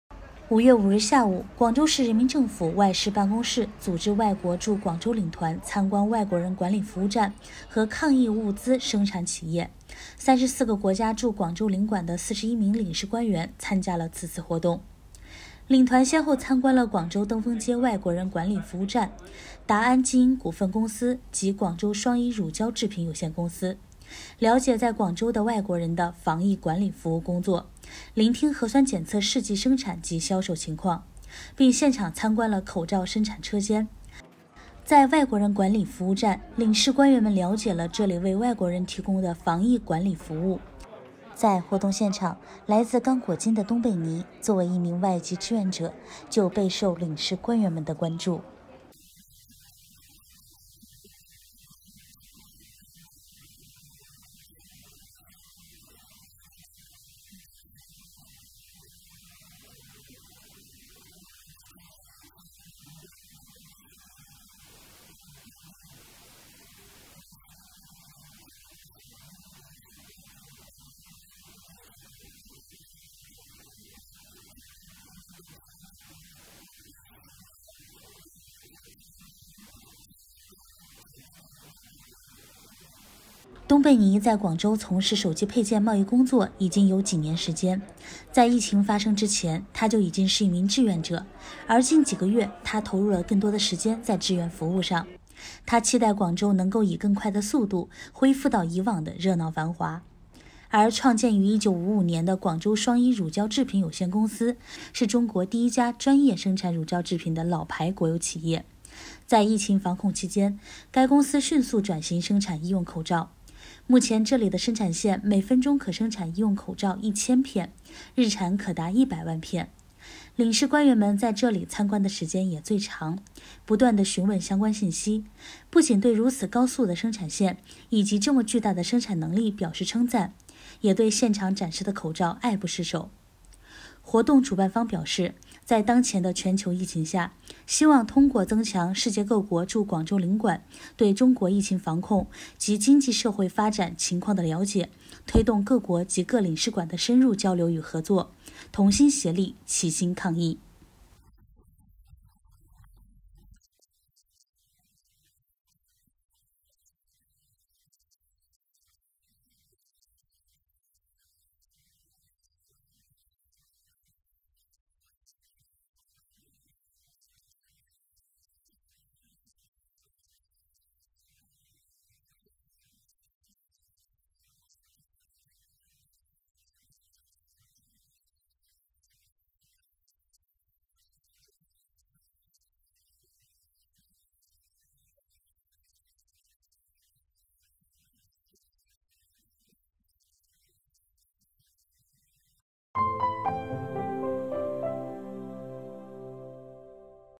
34国驻广州领团走访广州防疫一线
5月5日下午，广州市人民政府外事办公室组织外国驻广州领团参观外国人管理服务站和抗疫物资生产企业。34个国家驻广州领馆的41名领事官员参加了此次活动。领团先后参观了广州登峰街外国人管理服务站、达安基因股份公司及广州双一乳胶制品有限公司，了解在广州的外国人的防疫管理服务工作，聆听核酸检测试剂生产及销售情况，并现场参观了口罩生产车间。